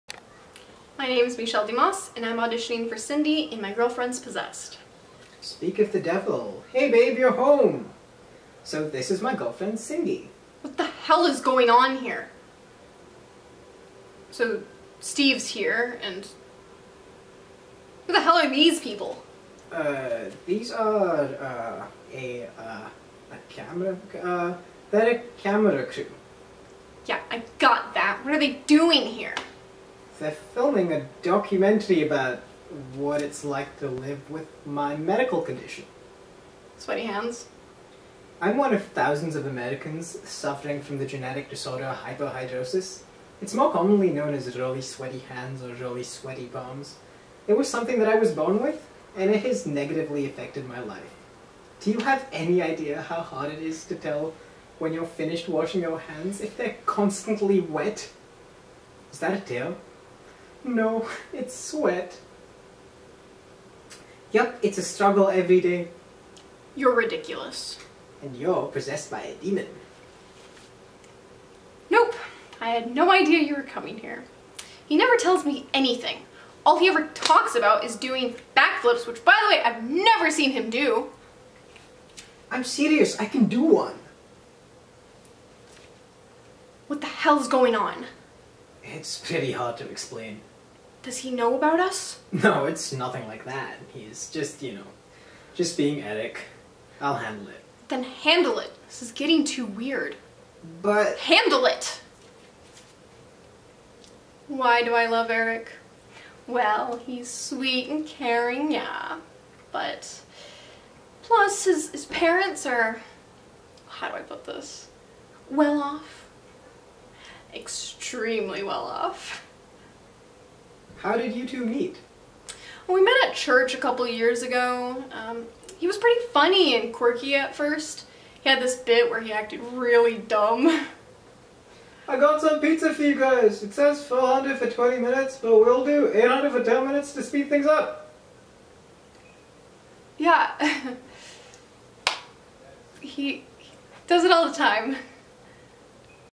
Self Tape for "My Girlfriend is Possessed"_Cindy
This is my self tape for the short film "My Girlfriend is Possessed". I am auditioning for the part of Cindy.

Synopsis: A documentary crew of 2 is hired by a man named Eric, who believes his girlfriend, Cindy, is possessed by a demon based off of a small amount of "evidence". After calling over his friend Steve Lowery to help, it becomes clear that Steve and Cindy are having an affair. Clear to everyone but Eric of course who decides to take drastic measures by hiring an exorcist, Father John, who is just as crazy as Eric. In an effort to sneak up on the demon, the group sets out to exorcise the demon with rather strange results.